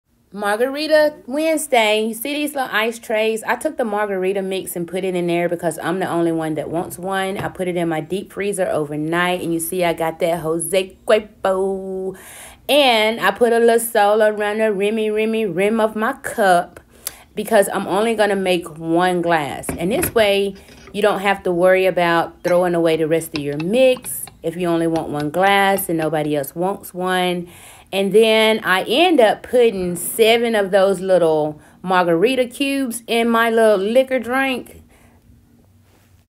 This is how I use frozen Margarita mix
This is how I keep from wasting my mix